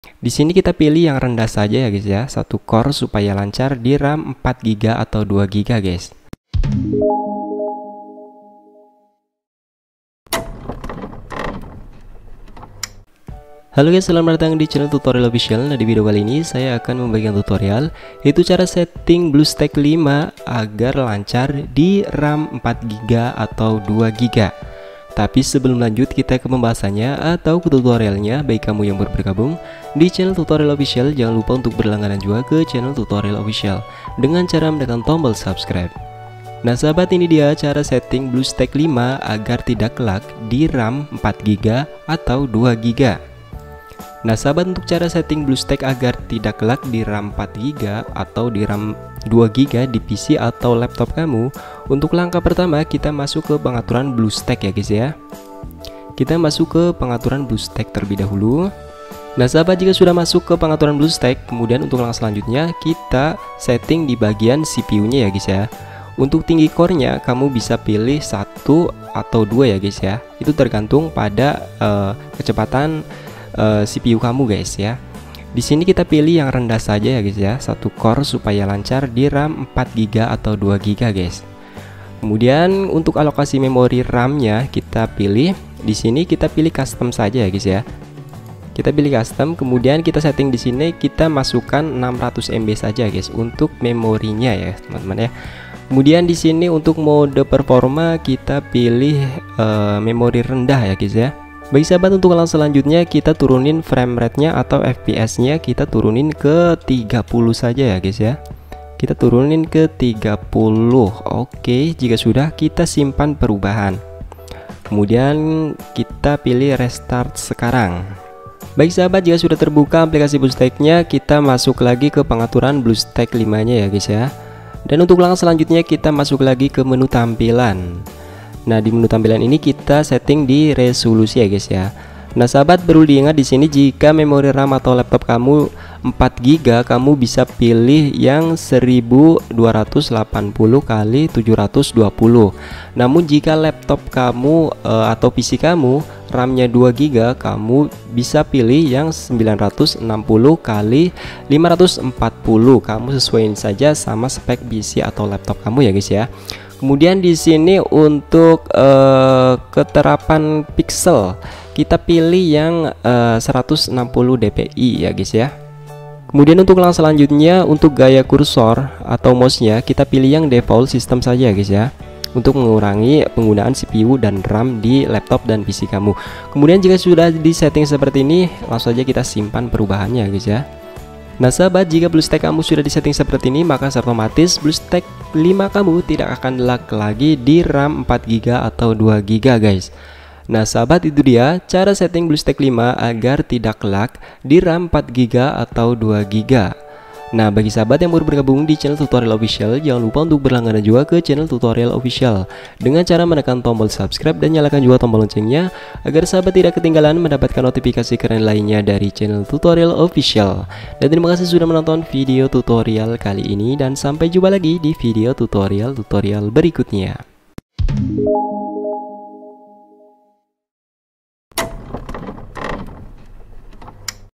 KHUSUS RAM 2GB/4GB - Cara setting bluestacks 5 agar ringan dan lancar | Tutorial Official
KHUSUS RAM 2GB/4GB - Cara setting bluestacks 5 agar ringan | @TutorialOfficial

Solusi Tepat Jika "BlueStacks 5 Terasa Lamban"
Bagaimana ini dapat membantu?
Panduan terperinci ini akan membantu kamu mengatasi permasalahan BlueStacks 5 yang terasa lamban lag dan patah patah atau masalah terkait kinerja, sehingga kamu dapat menikmati pengalaman bermain game yang lebih baik di BlueStacks 5.

Bagaimana cara mengatasi hal ini?
Silakan ikuti langkah-langkah yang diberikan di bawah ini untuk mengatasi kelambatan atau masalah terkait kinerja: KUSUS RAM 2GB/4GB - Cara setting bluestacks 5 agar ringan dan lancar | Tutorial Official

agar tidak salah paham jangan skip videonya dan tonton video tutorial nya sampai selesai.
Terimakasih.